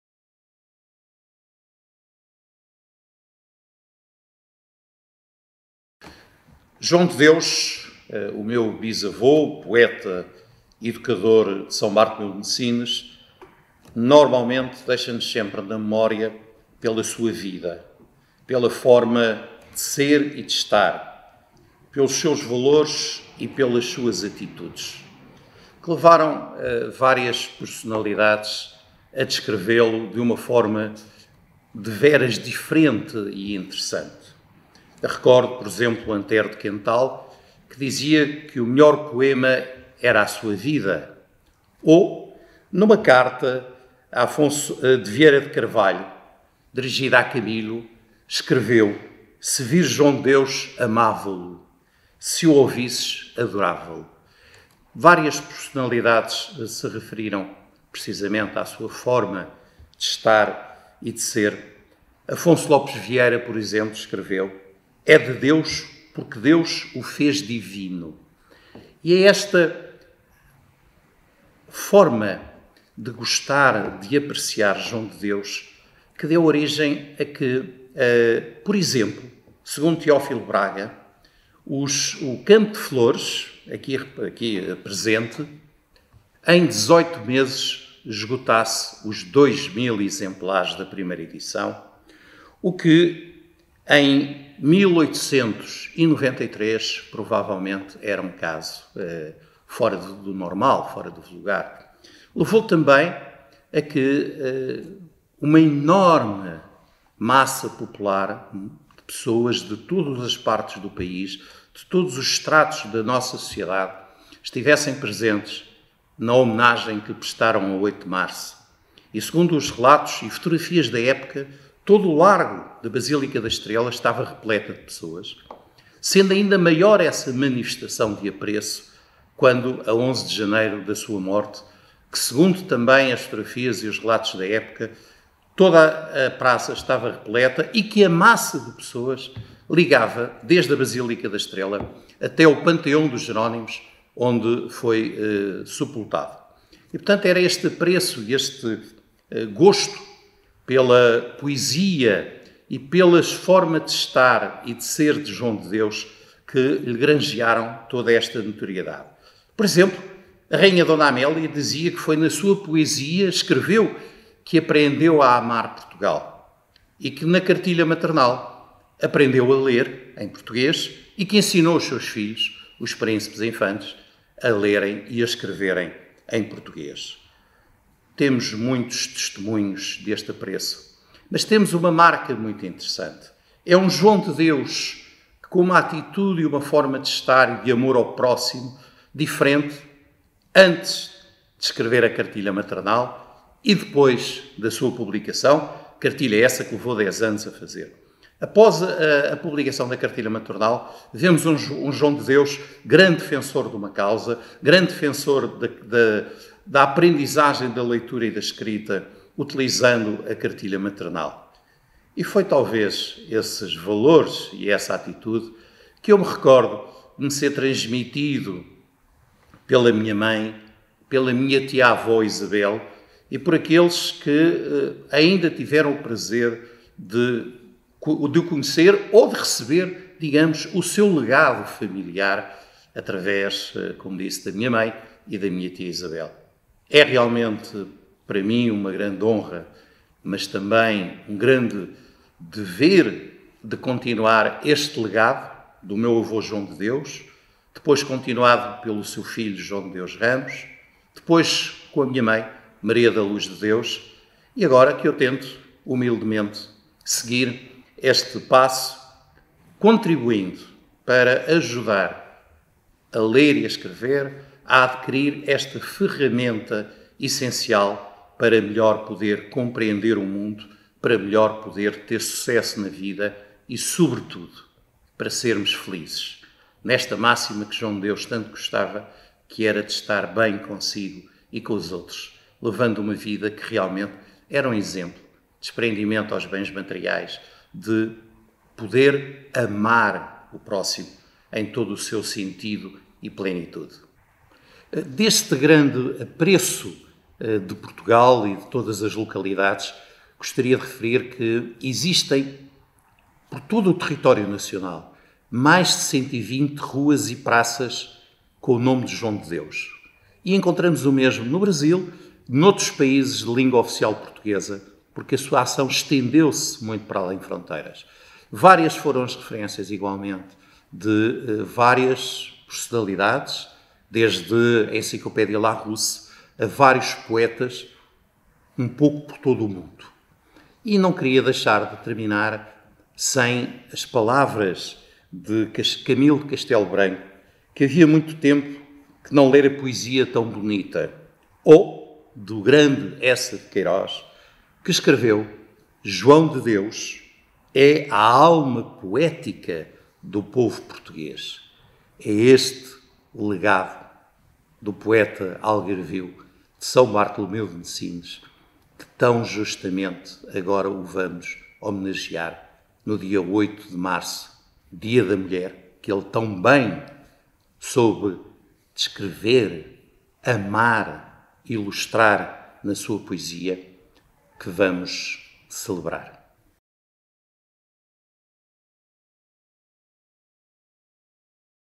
Quem é João de Deus? Conversa com António Ponces de Carvalho
Dia 8 de março assinalam-se 191 anos sobre o nascimento do poeta e pedagogo João de Deus. Homenageando o pai da cartilha maternal, o Município de Silves convidou o Presidente da Associação de Jardins-Escola João de Deus, Professor António Ponces de Carvalho (bisneto de João de Deus), para um momento de partilha sobre a vida e obra de João de Deus.